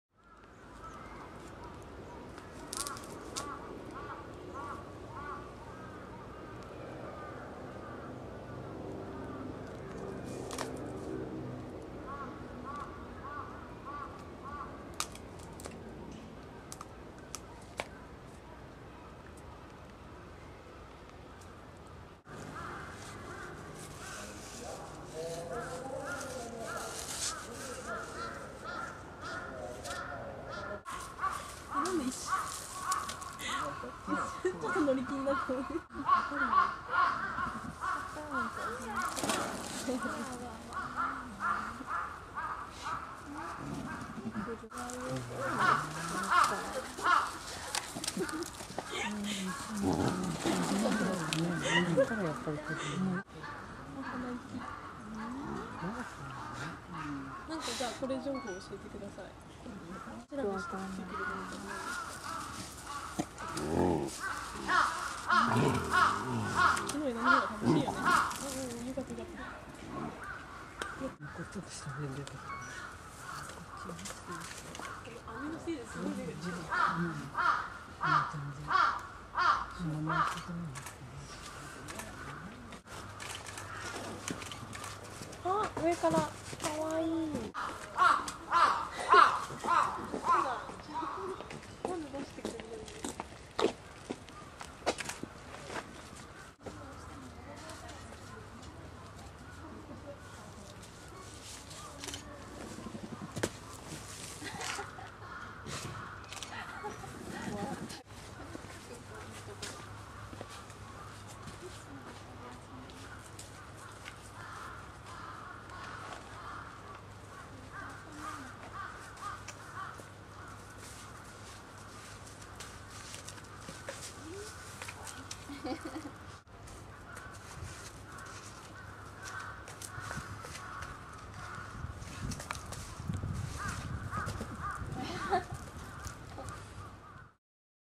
ユキヒョウの棒遊び ~Snow Leopard playing~
円山動物園のユキヒョウ、ヤマト(オス)とユッコ(メス)。
長い棒に夢中になって遊んでいますが、ヤマトが遊んでいると、必ずユッコがちょっかいをかけにきます。
横から走ってきたり、上から飛び降りて来たり。
平成21年5月2日に生まれ　札幌円山動物園 09年11月27日撮影)
ブログ「北の暮らし ~札幌・宮の森から~」内で、ユキヒョウ物語も毎日更新中。